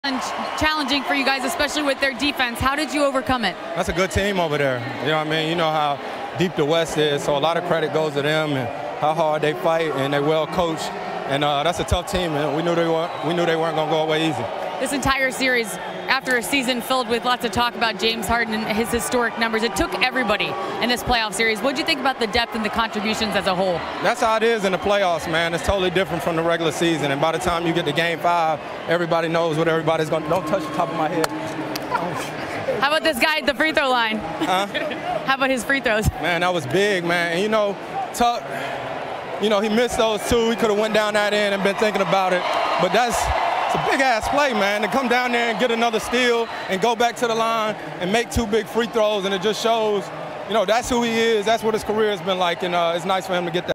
CP3 Talks Intensity Of Playoffs, PJ Tucker HILARIOUSLY Interrupts Touching Paul's Head
A veteran like CP3 knows that playoff intensity is like no other, paying respect to the Jazz who took them to 6 games. Best part of interview comes around the :28 mark when PJ Tucker rubs the back of Paul's head.